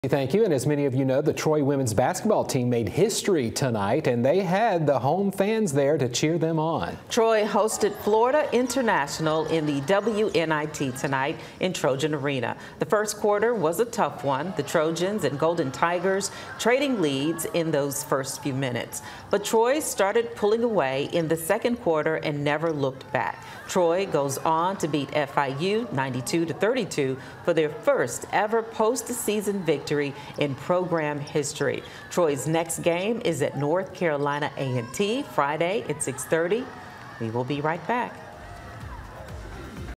Troy women's basketball team makes history with first-ever postseason victory
Troy women's basketball makes history with their first-ever postseason win in program history.